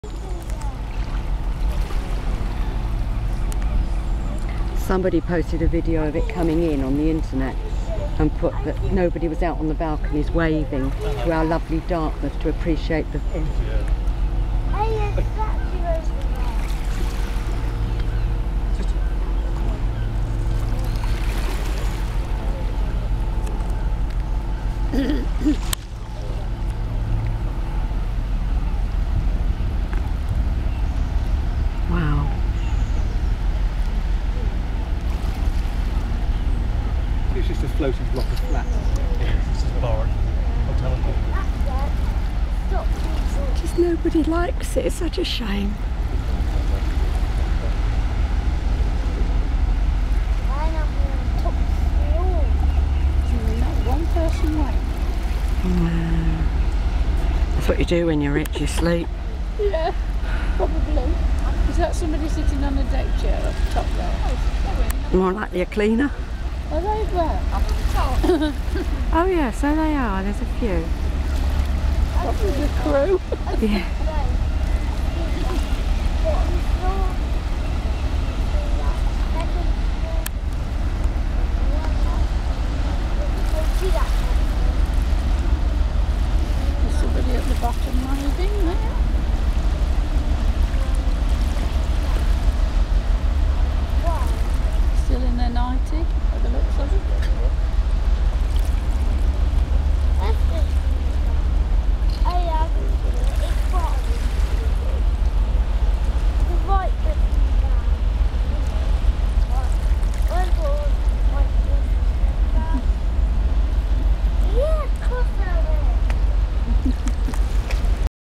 The World leaving Dartmouth, Devon, UK . 29 July 2010 (4 of 5)
On 29th July 2010  at  08.00  'The World'  left Dartmouth, Devon, UK.

A local journal said  - when looking across the river The World would block out the town of Kingswear on the opposite side - and it did. The World's huge.

It had stayed since 27 July 2010 and during its stay it attracted many visitors which I hope was good for Dartmouth's businesses.

Please excuse the idle chatter in the background of this video as I couldn't find a spot on my own and I had to share a jetty with about five other people.